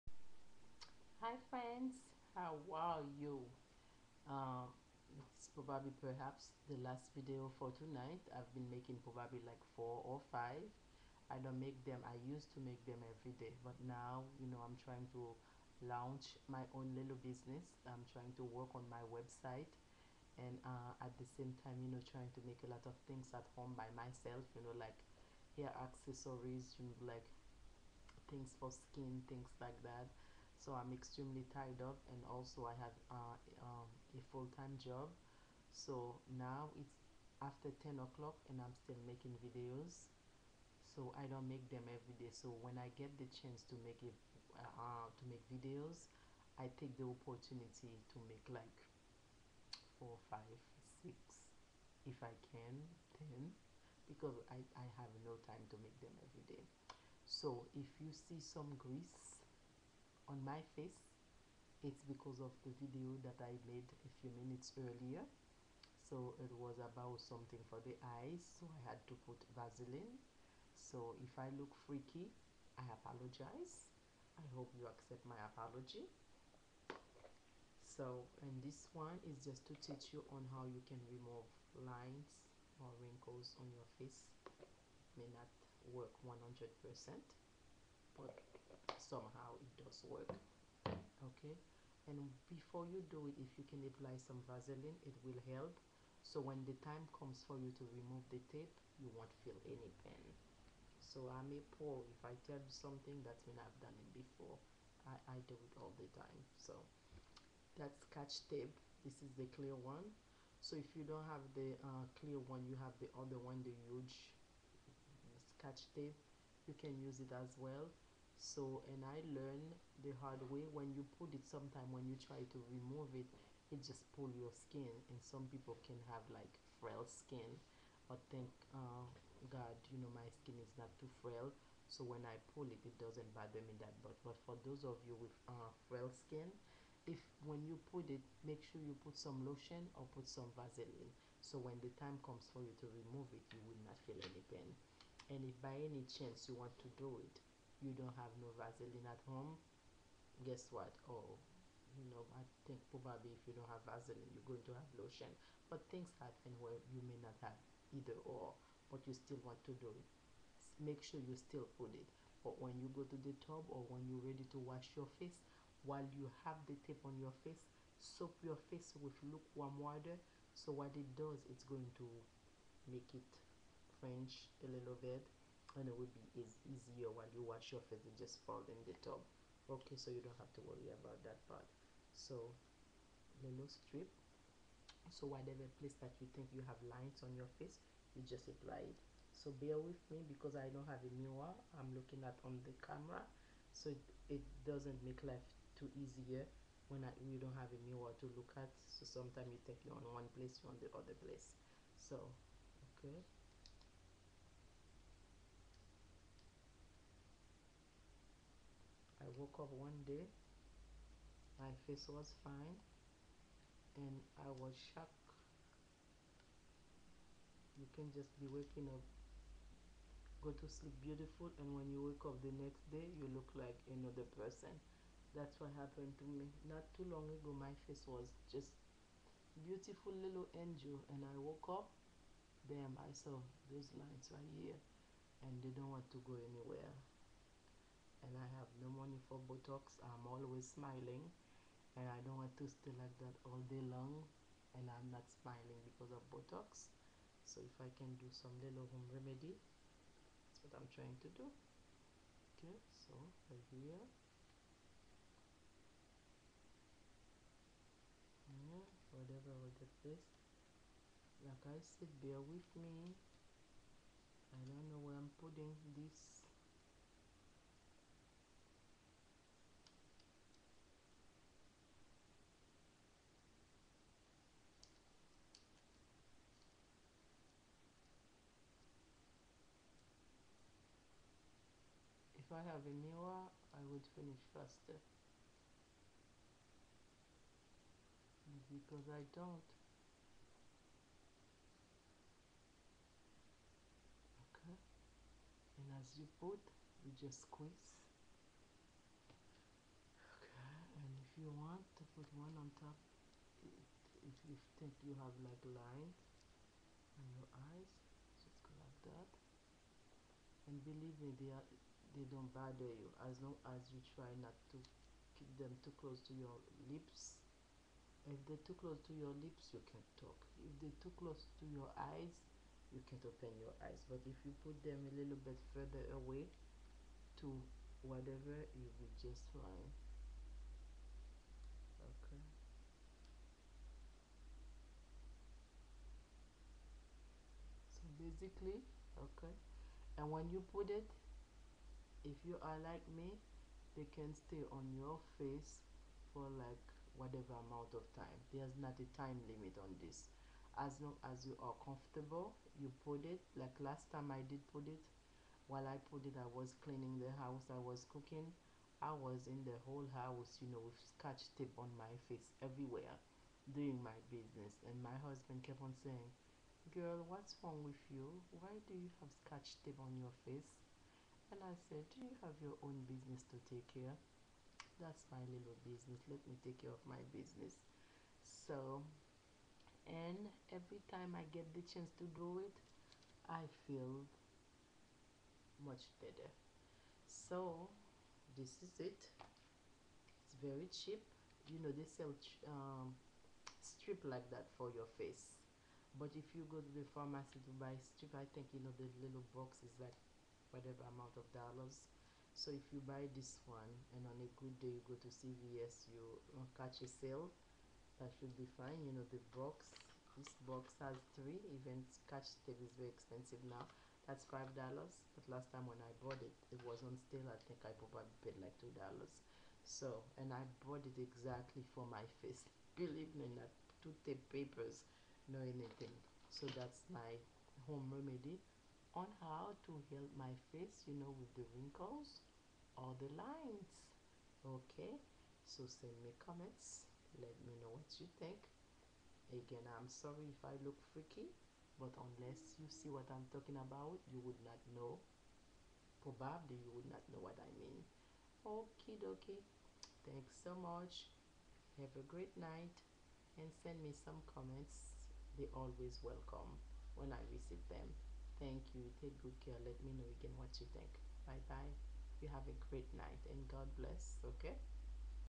HOW TO REMOVE LINES ON YOUR FACE
SCOTCH TAPE, VASELINE OR LOTION